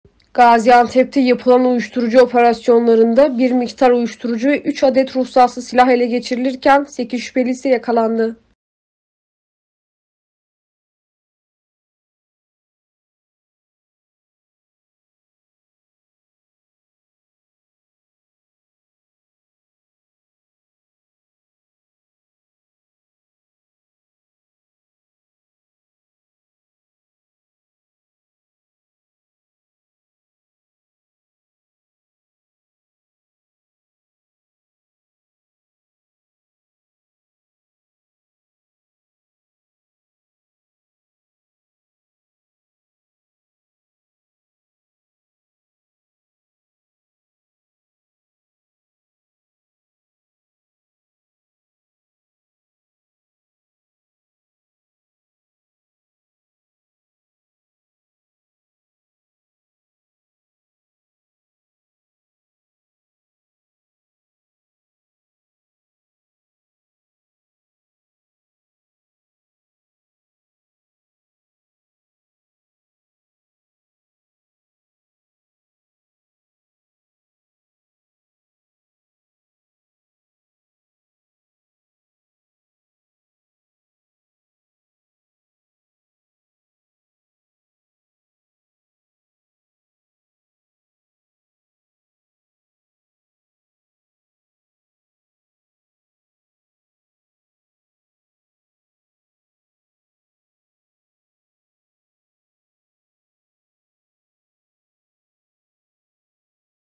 Uyuşturucu operasyonlarında 8 şüpheli yakalandı
Gaziantep’te yapılan uyuşturucu operasyonlarında bir miktar uyuşturucu ve 3 adet ruhsatsız silah ele geçirilirken, 8 şüpheli ise yakalandı.